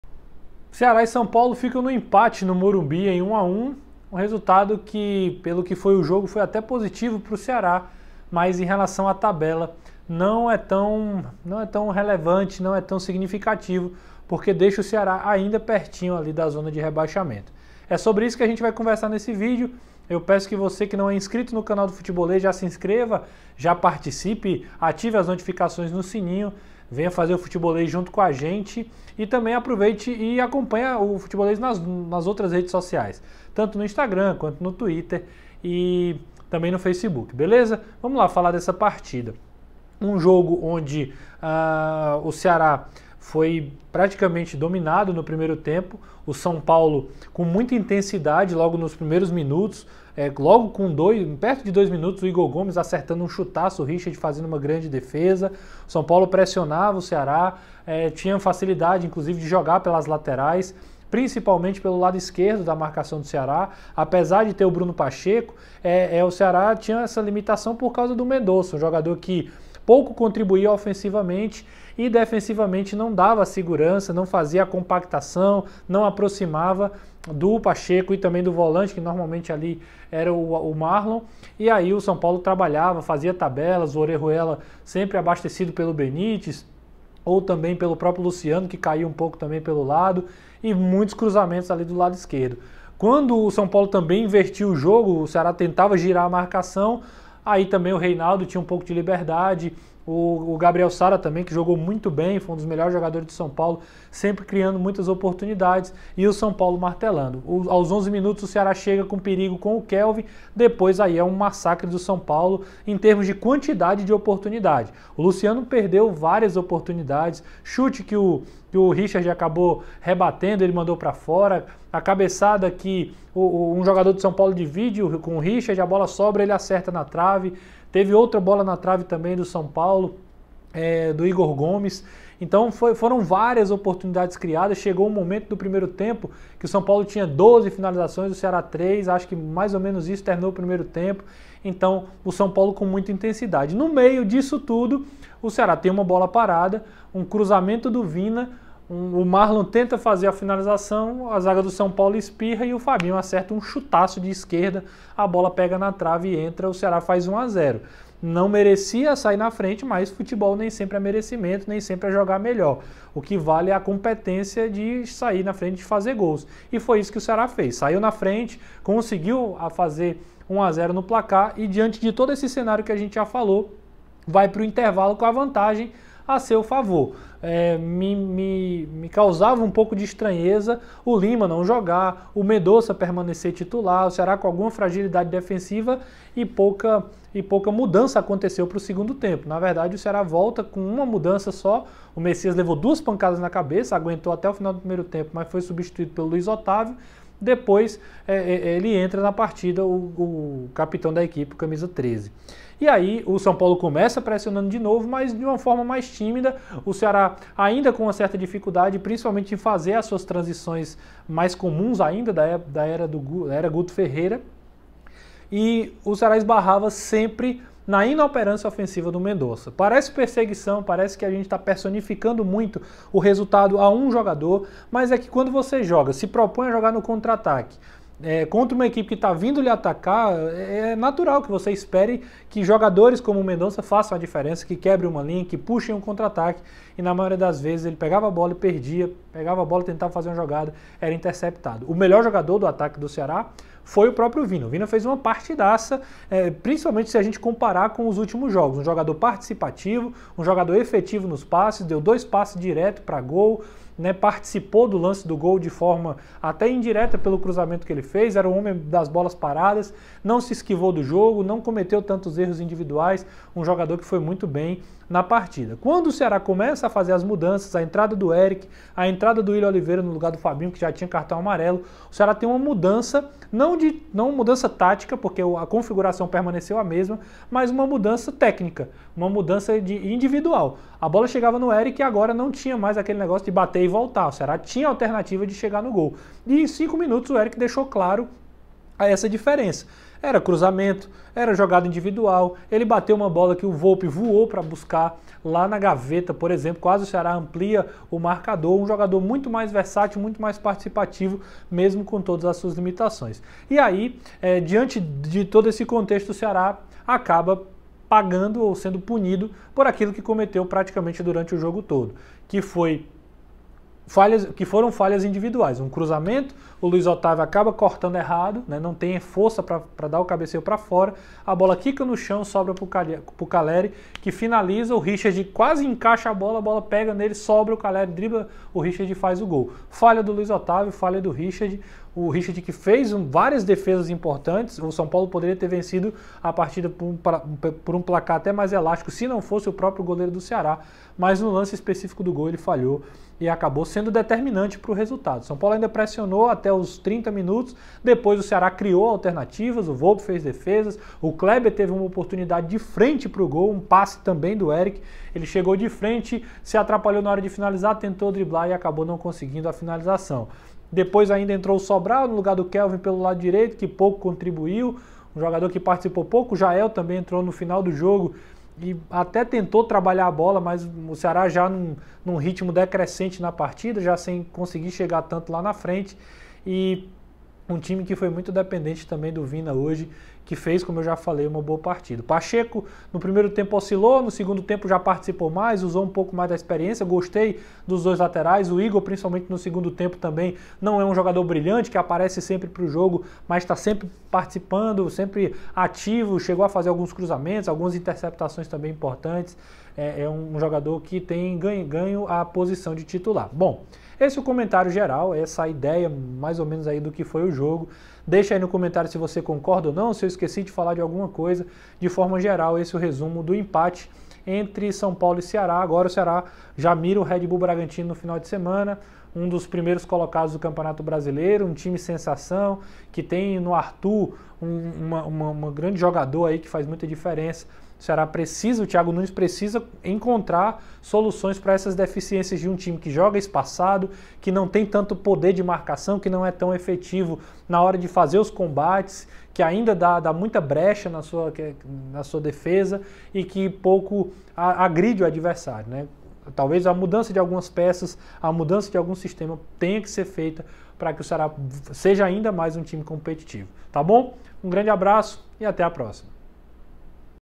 SÃO PAULO 1 X 1 CEARÁ - FABINHO FAZ UM GOLAÇO, MAS VOVÔ EMPATA E SEGUE SEM VENCER FORA DE CASA
Ceará sai na frente com Fabinho, mas cede o empate após falha da defesa. Quem comenta o jogo é Renato Manso.